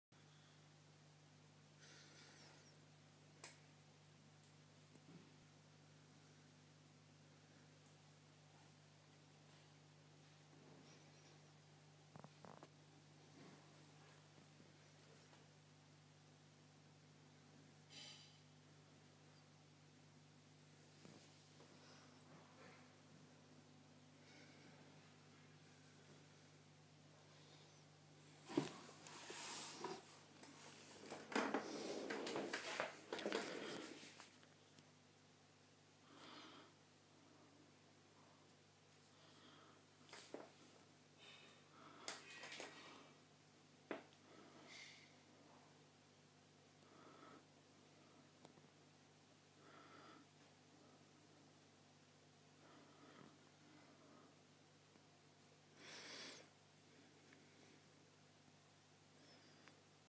Mes poissons complètement fou
Mes piranha en train de manger leur repas